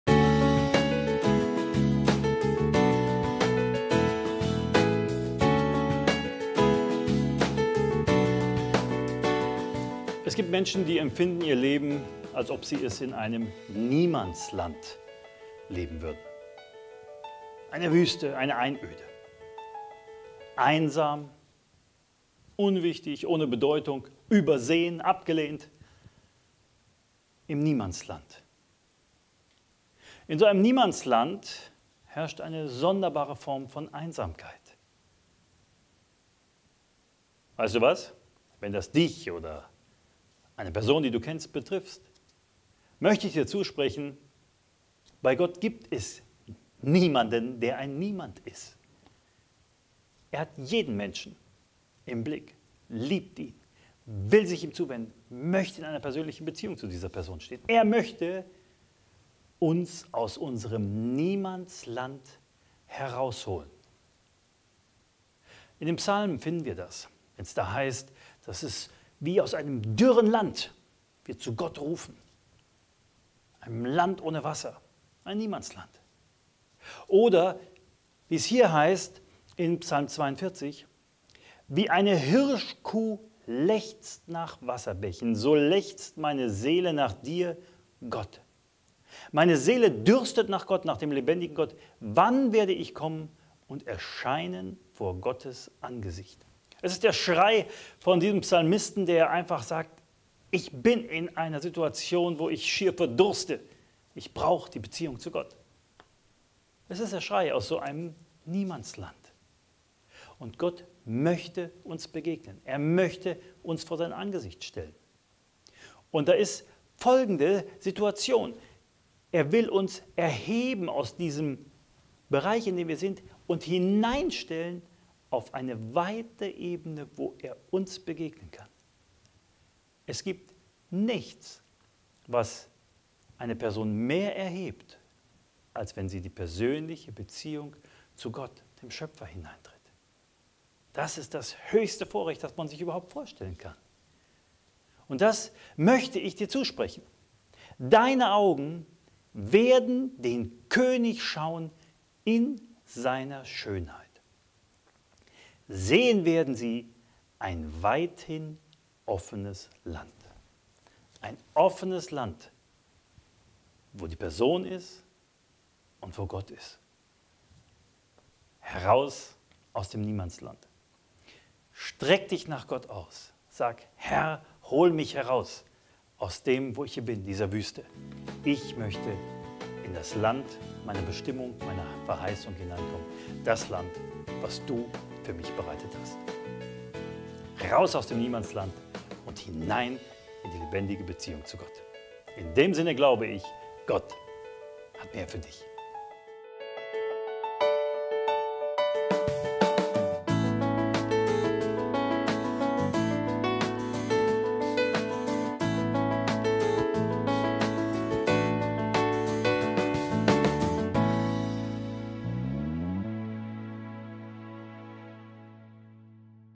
Niemandsland
Manche Menschen empfinden ihr Leben wie ein Niemandsland. Ein Land, in dem Einsamkeit herrscht, das leer und öde ist. Sie werden übersehen, nicht wahrgenommen, fühlen sich bedeutungslos. Und manch einer denkt sich: Eigentlich ist es egal, ob es mich gibt oder nicht, es kümmert sowieso niemanden.
Doch ich kann dir versichern: Gott will dich aus deinem Niemandsland herausholen. Bei ihm gibt es niemanden, der ein Niemand ist.

Wie eine Hirschkuh lechzt nach Wasserbächen, so lechzt meine Seele nach dir, Gott! Meine Seele dürstet nach Gott, nach dem lebendigen Gott: Wann werde ich kommen und erscheinen vor Gottes Angesicht?
Psalm 42,2-3

Was bist du so aufgelöst, meine Seele, und was stöhnst du in mir? Harre auf Gott! – denn ich werde ihn noch preisen, das Heil meines Angesichts und meinen Gott.
Psalm 42,12

Deine Augen werden den König schauen in seiner Schönheit, sehen werden sie ein weithin offenes Land.
Jesaja 33,17